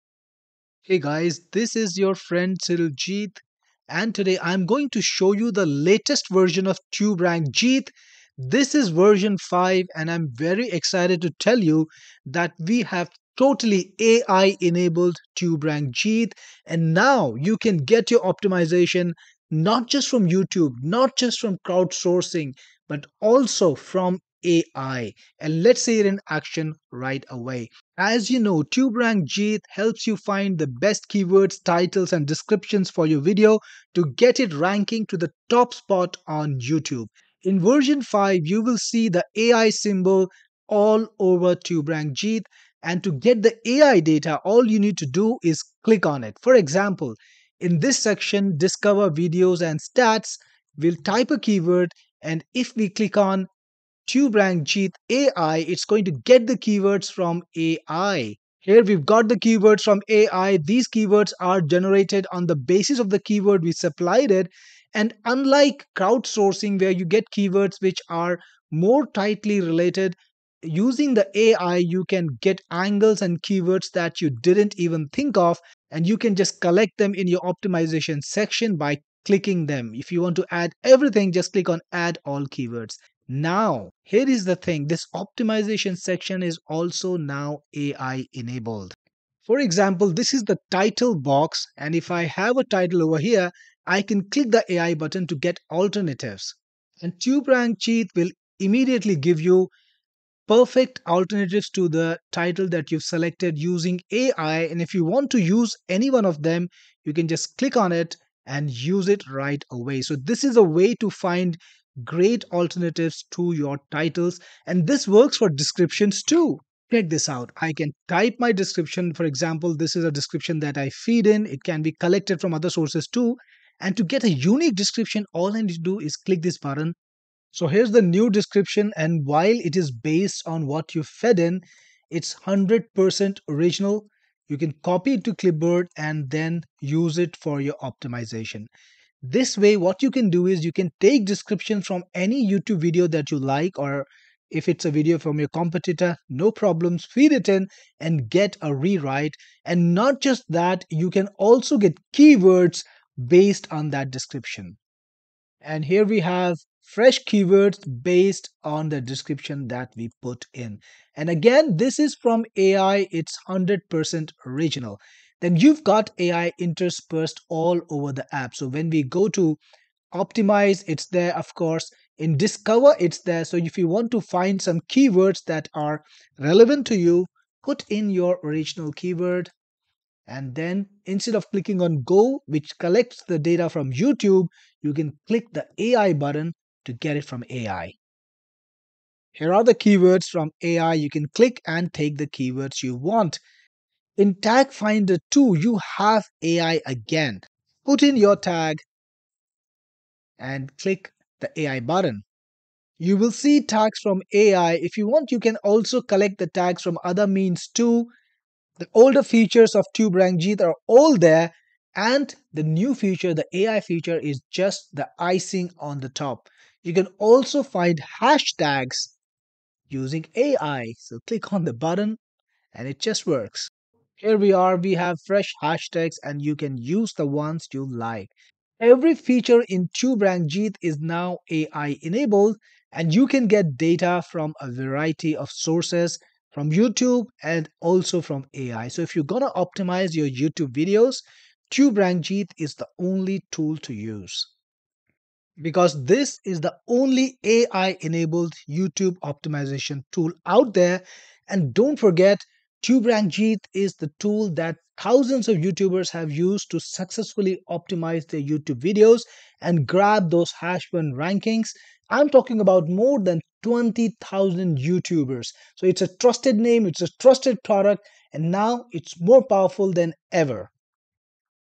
Tuberank Jeet AI - Rank your videos on YouTube with AI Technology
Tuberank Jeet is now AI enabled. With Version 5, this is the first YouTube ranking software to have built-in AI.

Watch this video to see how Tuberank Jeet can help you find the best Titles, Descriptions, Keywords and optimizations for your YouTube videos.